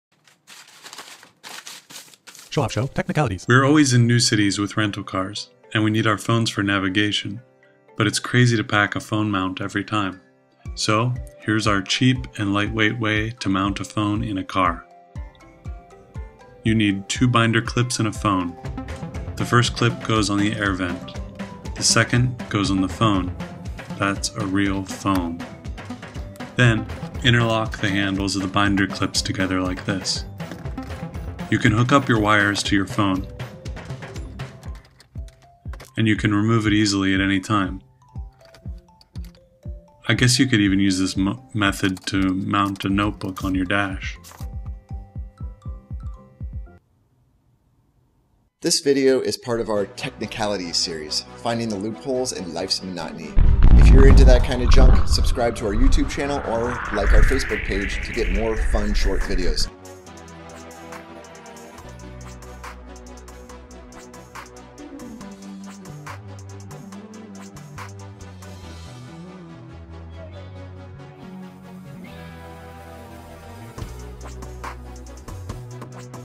Simple Phone Mount for Your Car
Here's how to mount your cellphone in nearly any car without a store-bought dock. It just uses binder clips available at most stores.